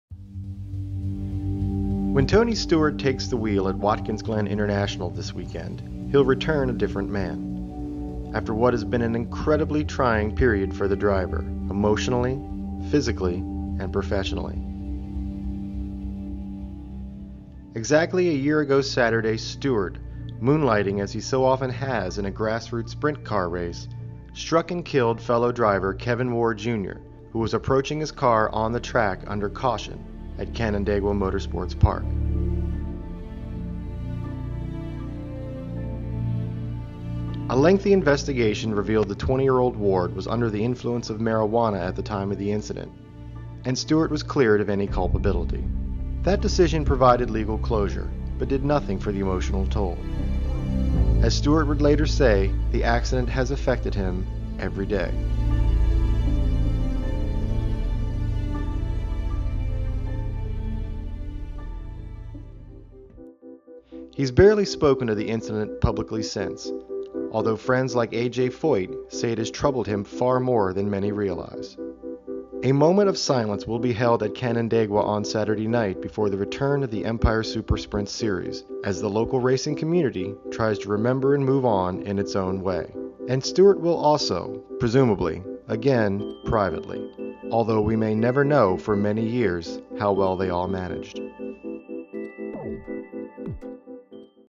Tony Stewart moving forward one year after deadly accident
USA Today Sports' Brant James explores the one-year anniversary of Tony Stewart's tragic accident in New York, and how the episode has affected Stewart on and off the track.